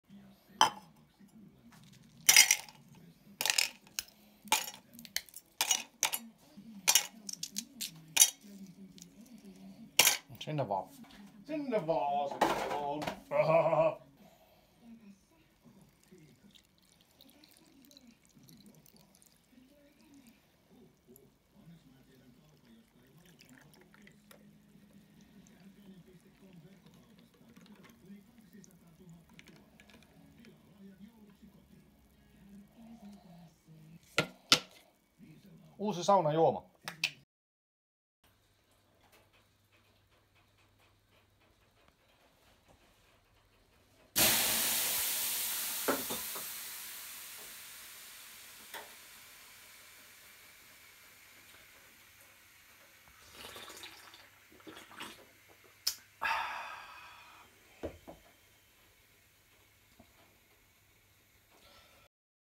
New Sauna Drink Part 1, 4K UHD
Now that I haven't been drinking beer for quite while, I wanted to try new sauna drinks. So this is the Paulig's Frezza Forte Double Trouble - Double Shot Espresso & Milk, served in a metal cup filled with ice.